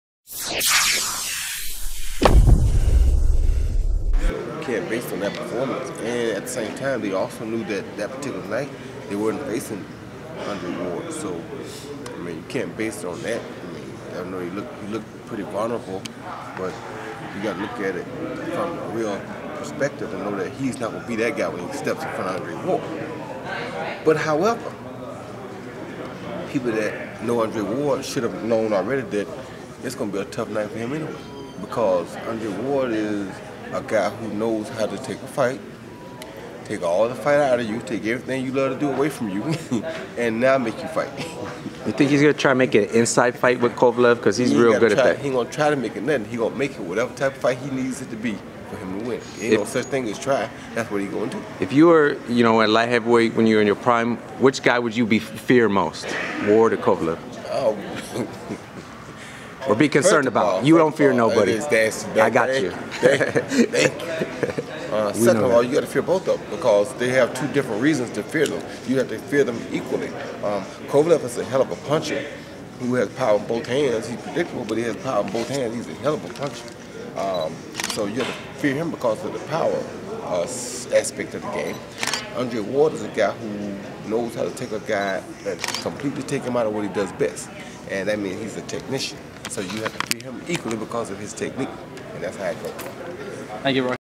Roy Jones Jr. talks Sergey Kovalev vs. Andre Ward and facing each man in his prime
speaks with four-division world champion and living boxing legend Roy Jones Jr. as he speaks on the upcoming light heavyweight duel between lineal champion Sergey Kovalev and former super middleweight king Andre Ward.